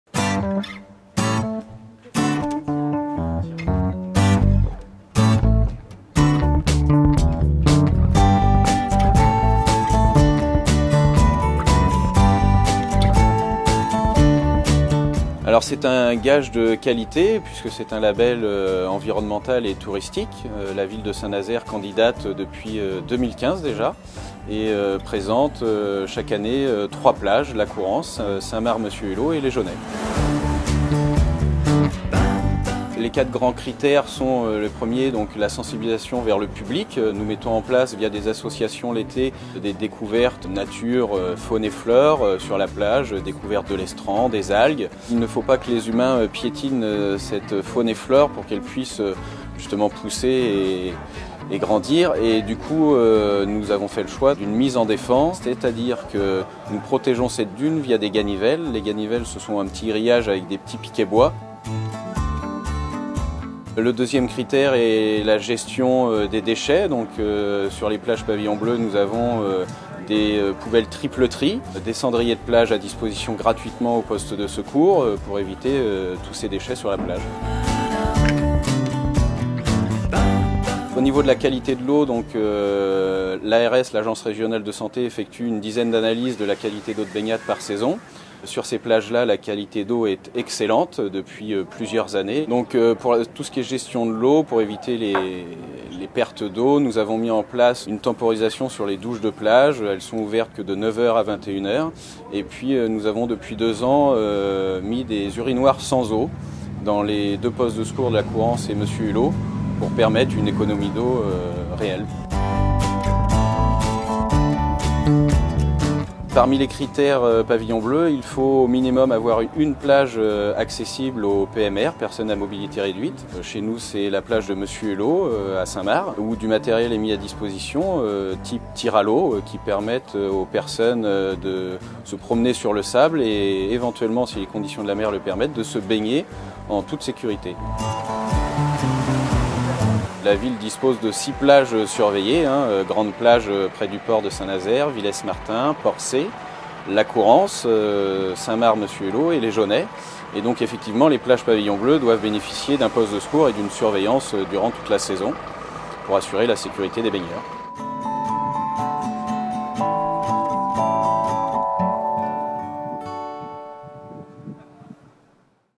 Les plages Pavillon bleu à Saint-Nazaire
Trois plages sont labellisées Pavillon bleu à Saint-Nazaire : la Courance, les Jaunais et Monsieur Hulot.
Elles répondent à des critères de qualité de l'eau, mais pas seulement.